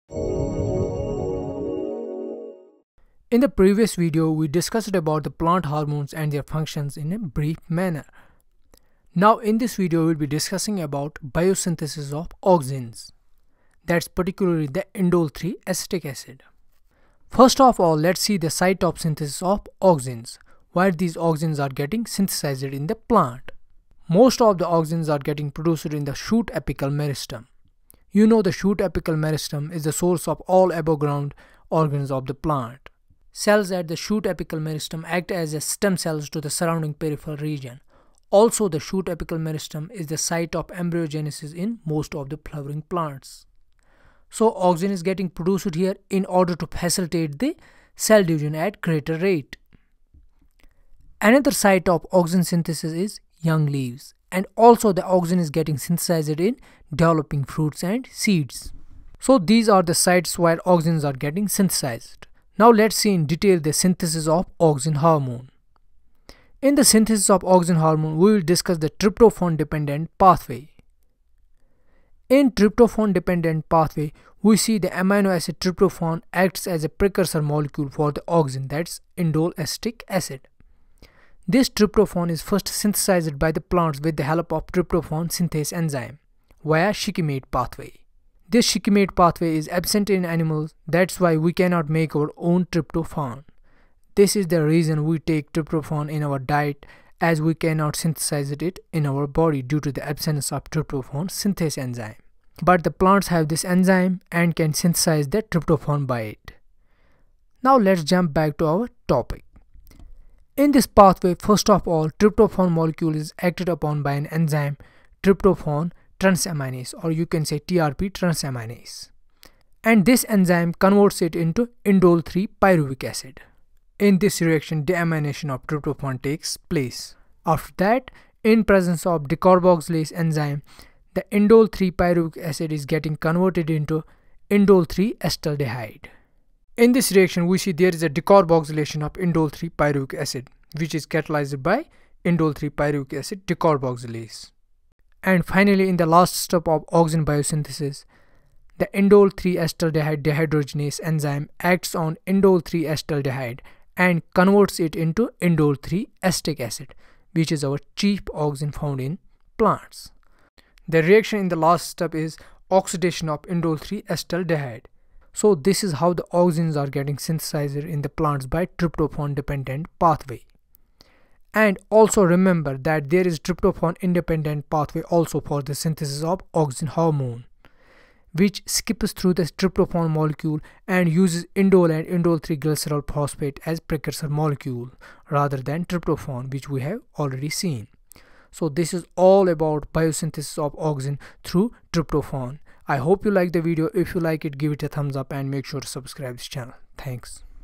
Biosynthesis of Auxin
In this Video we have discussed the Biosynthesis of Auxin mediated via Tryptophan Dependent Pathway.
The Tryptophan in plants is produced through Shikimate Pathway and is used to synthesize  folates and aromatic amino acids.
The Tryptophan Acts as a precursor molecule for Auxin Biosynthesis.